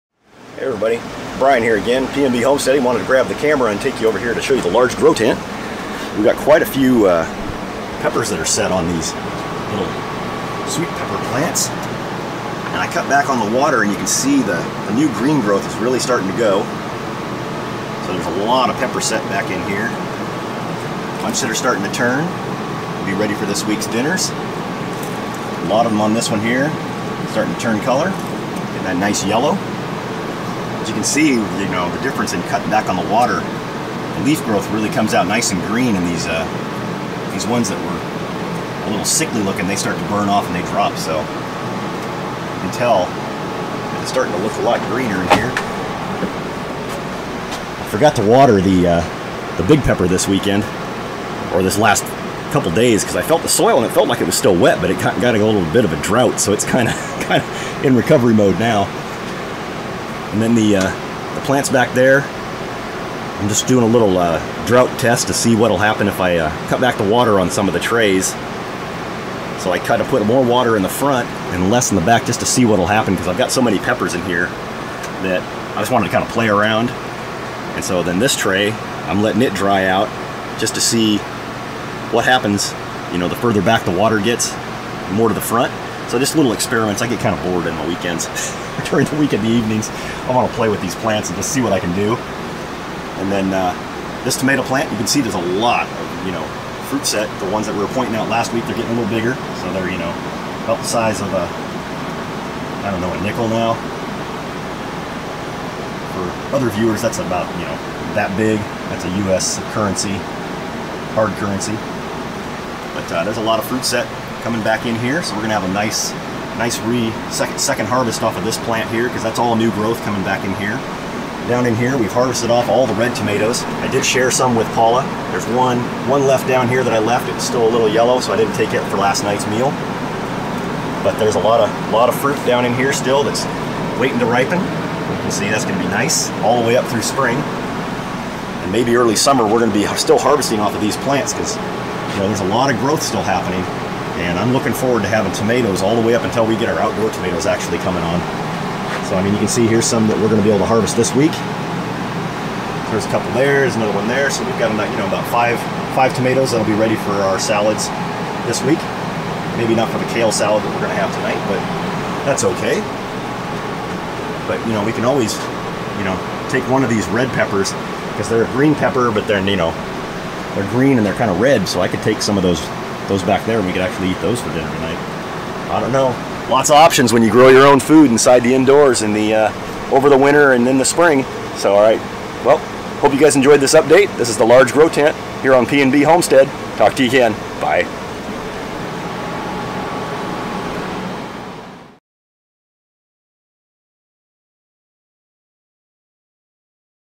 Large grow tent update 2019 week 12
Hi Everyone, the always dependable large grow tent is giving out peppers and tomatoes on a regular basis now. Enjoy the video update! :-)